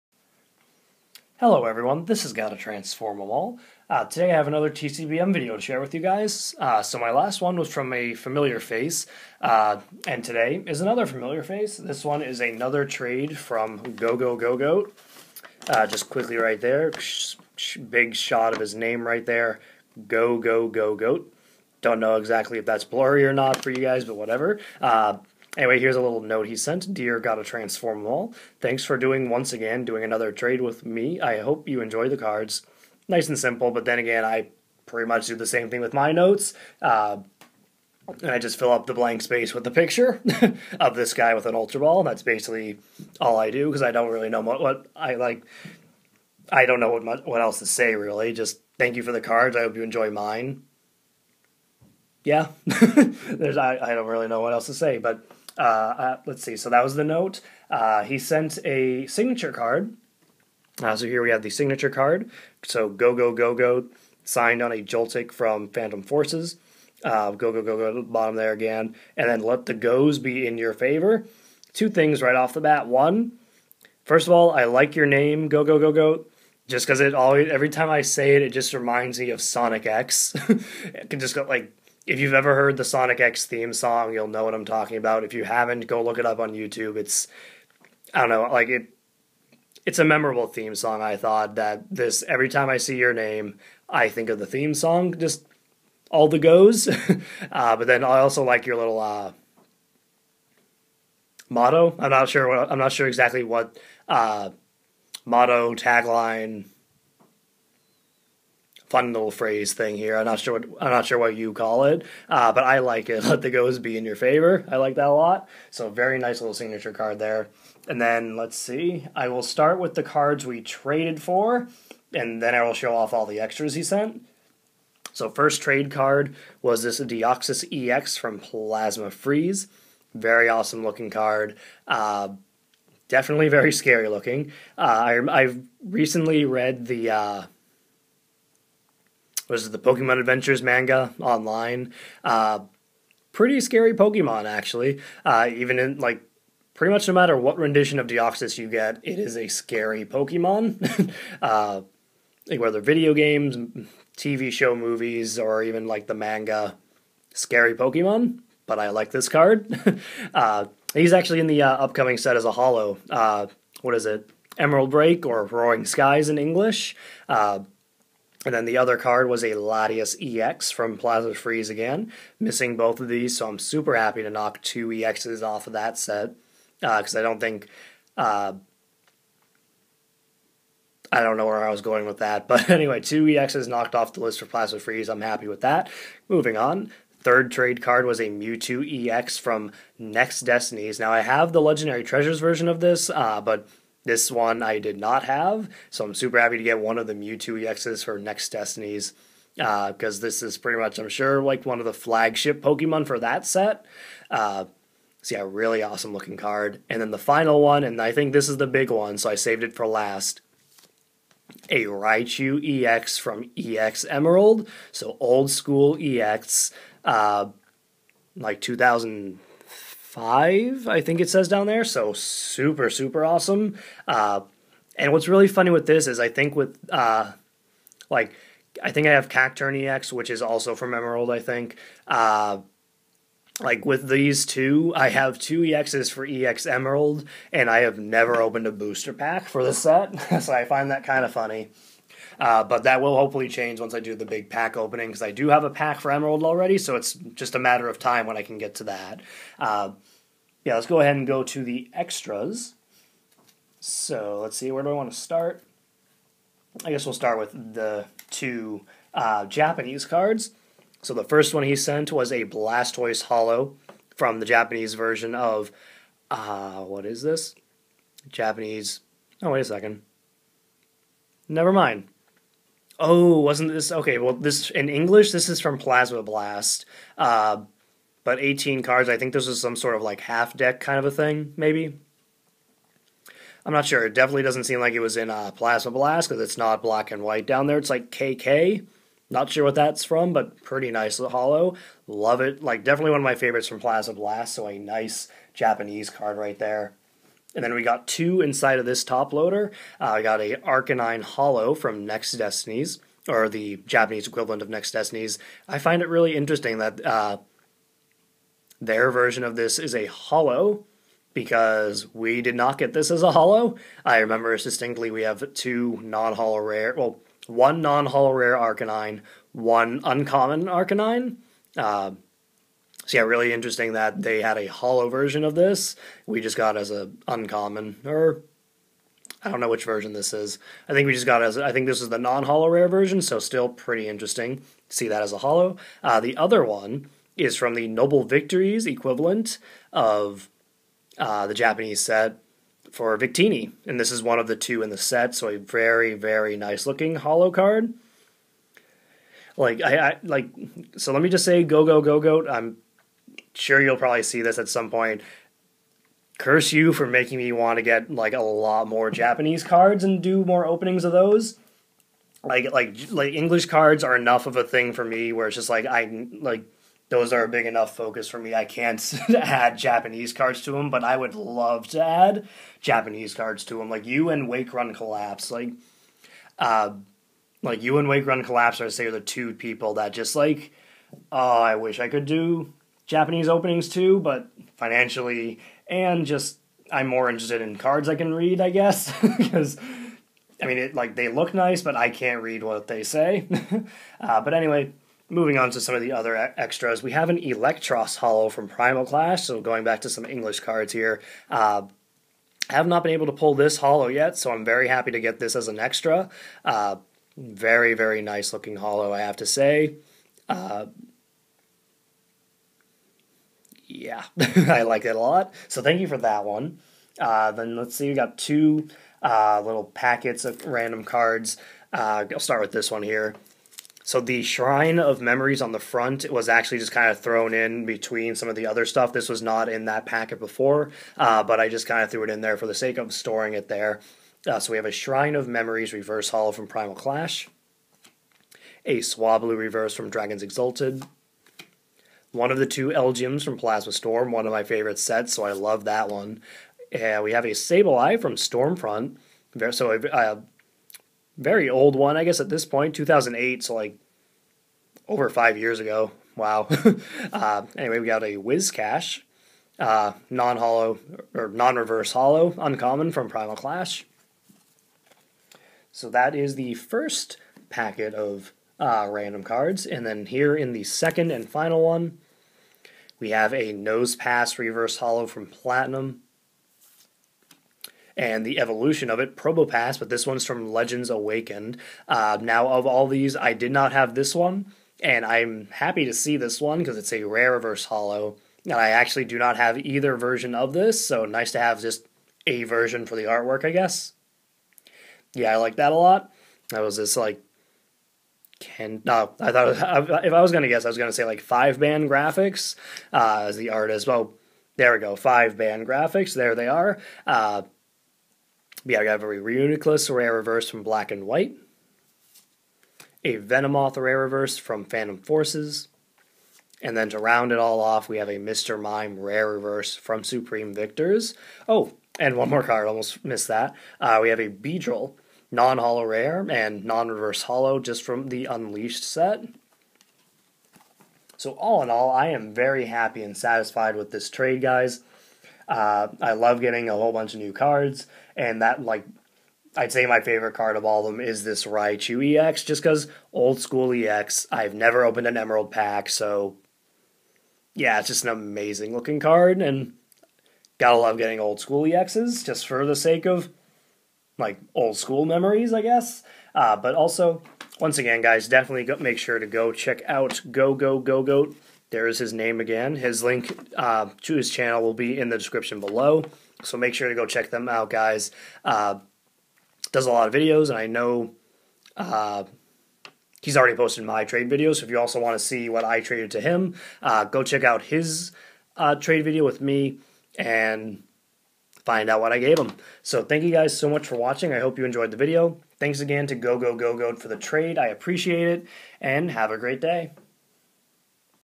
TCBM: Gogogogoat (4th Trade)
I need to apologize to gogogogoat, I did not post this as I said I would last week and I am just now realizing it was not uploaded. This is the 4th trade I've done with him and I've had no problems with any of them, so please go check him out guys!

Gogogogoat's channel: